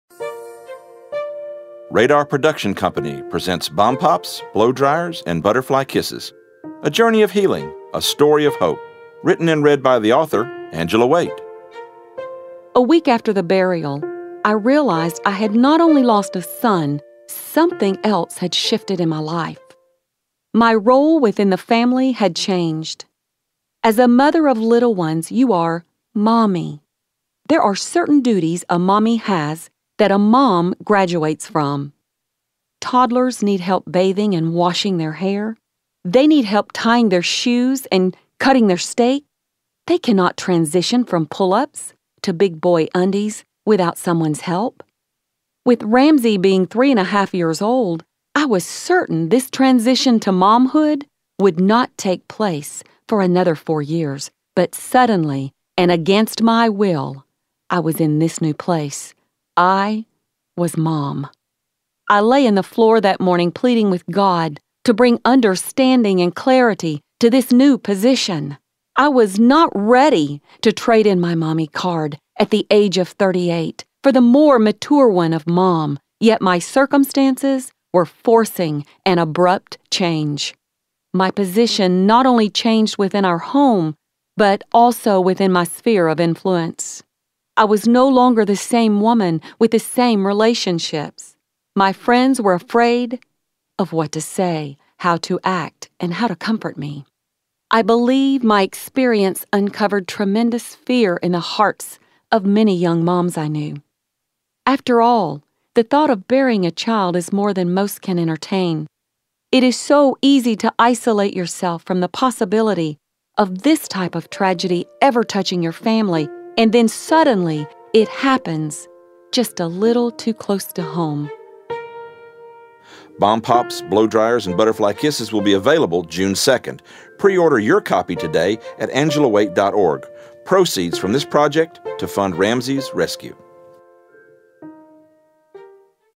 bomb pops, blow dryers & butterfly kisses (promo)
On Friday, May 26, 2000, the Waite’s youngest son, Ramsey, drowned in their backyard pool at the age of three and a half. What had been a perfect day—indeed a financial miracle had happened just that morning—turned into a family’s worst nightmare.
As the Waites fought to make sense of the tragedy, Angelia spiraled into a deep depression, further burdened by a life-threatening disease, and by the hurtful words spoken by those in her community. Her family needed room to breathe, to heal. And Angelia needed answers: Why did this happen? Where were you, God? He answered by orchestrating the sale of their home, by sending Angelia on a mission trip where she was healed of her disease, by calling her into ministry, and by strengthening their family bond tenfold.
Today the Waites are all thriving, blessed beyond measure—thanks to a loving and merciful God who takes the messes of our lives and turns them into messages of hope.